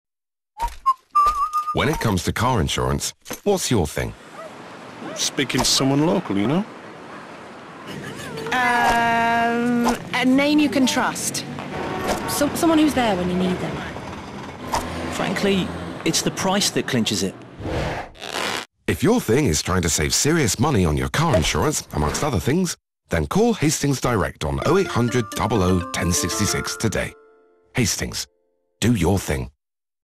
Hastings Direct classic TV Advert February 2008
A classic advert from Hastings Direct dating back to 2008.

At Hastings Direct we still offer refreshingly straightforward car insurance.